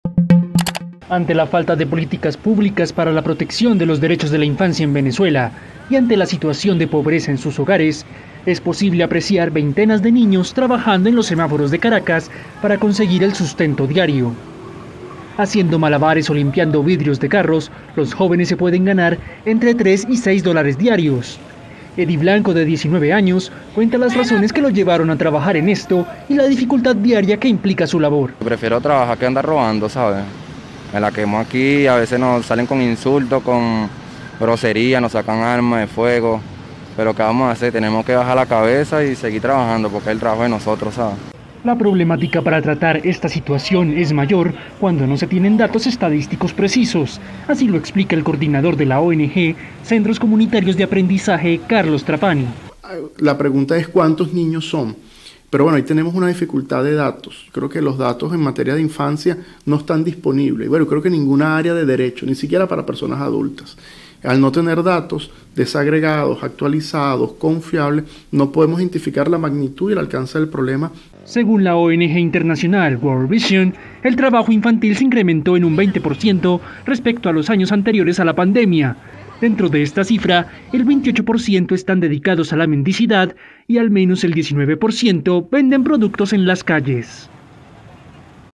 Niños pobres en Venezuela se ven obligados a trabajar en las calles para sobrevivir
La falta de políticas públicas en torno a la protección de los derechos de la infancia en Venezuela ha derivado en la vulneración de los derechos de los menores, muchos de los cuales se ven obligados a salir a las calles para trabajar y conseguir el sustento diario, aseguran organizaciones y expertos.